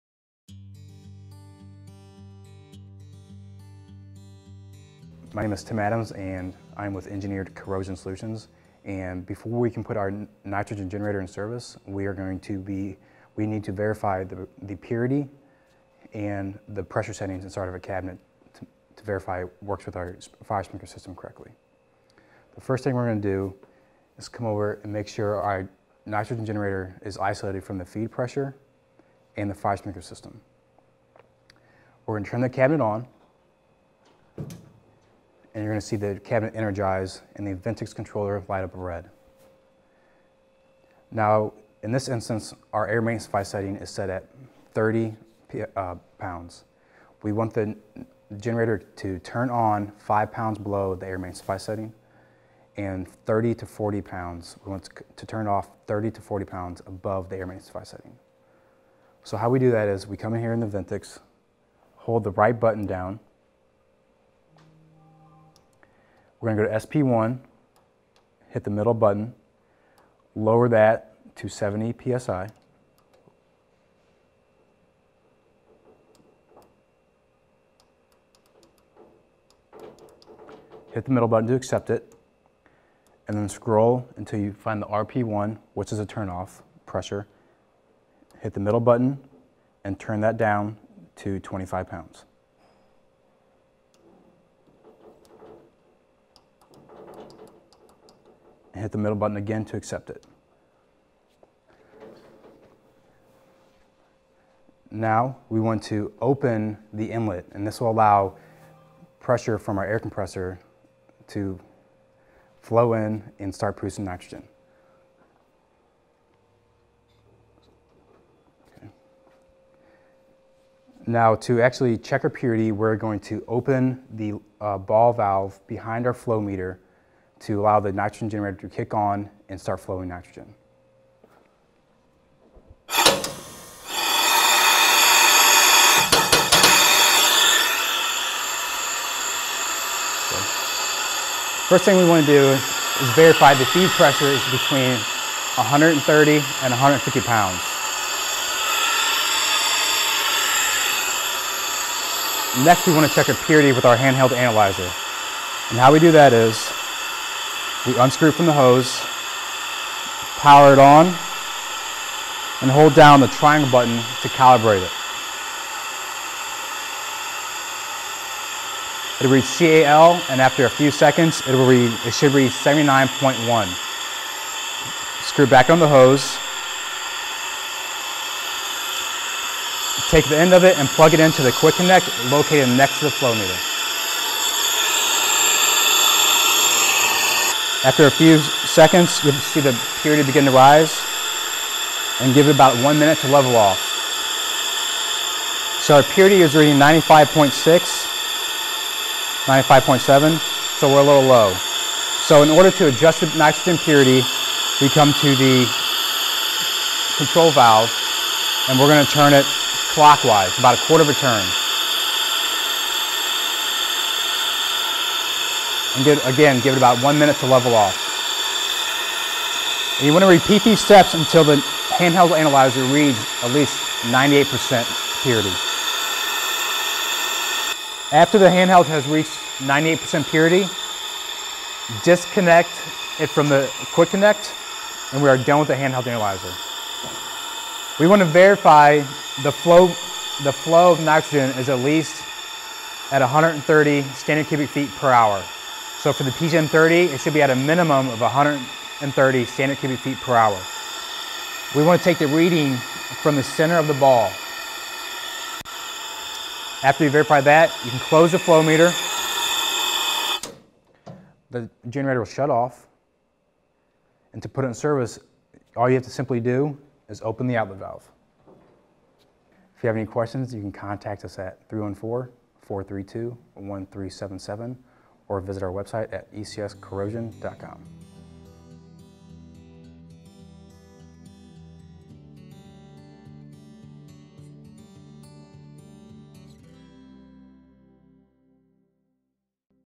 PGEN-30 Walkthrough: Nitrogen Generator Pre-Startup
A walkthrough of pre-starting your PGEN-30 unit.